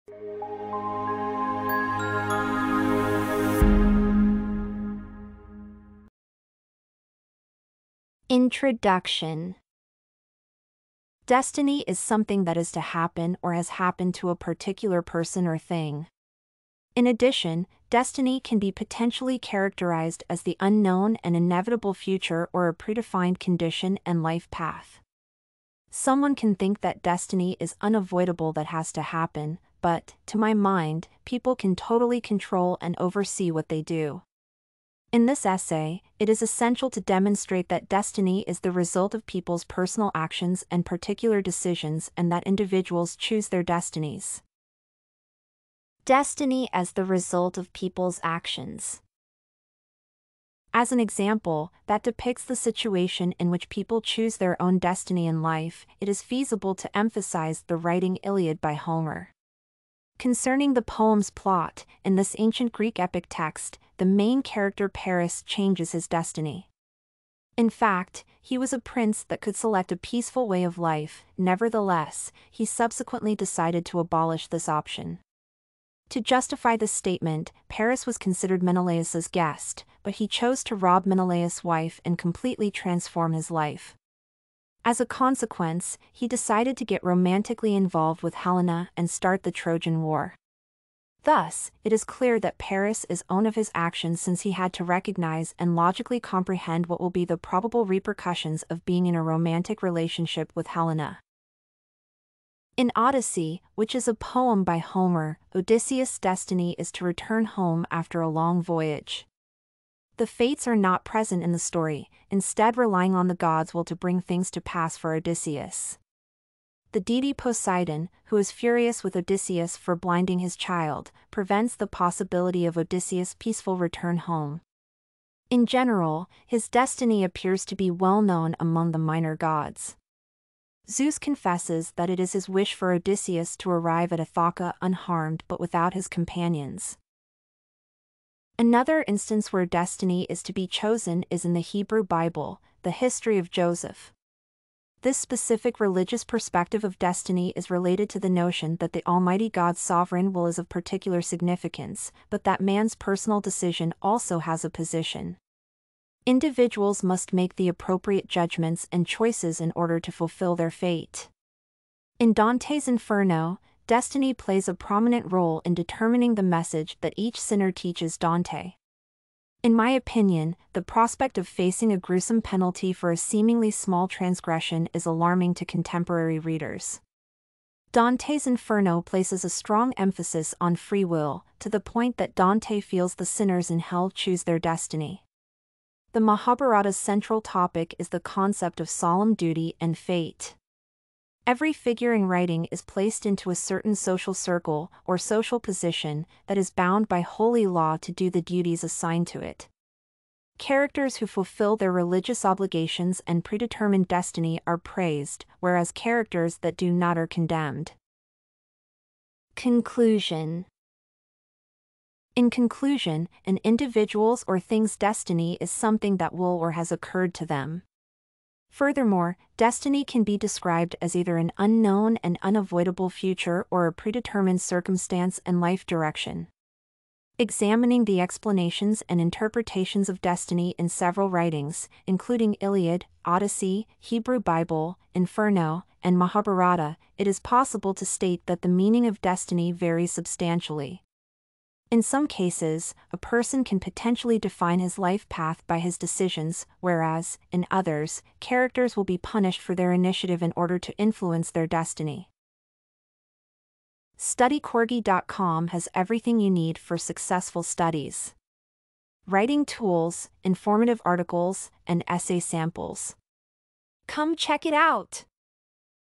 What Is Destiny? Literary Works Answer - Essay Example
📄 Essay Description: In this essay, it is essential to demonstrate that destiny is the result of people’s personal actions and particular decisions and that individuals choose their destinies.

✔️ Topics: Literature
✔️ Language: 🇺🇸 English
✔️ Words: 599

🔍 References:
Kelly, Adrian, and Christopher Metcalf. Gods and Mortals in Early Greek and Near Eastern Mythology. Cambridge University Press, 2021.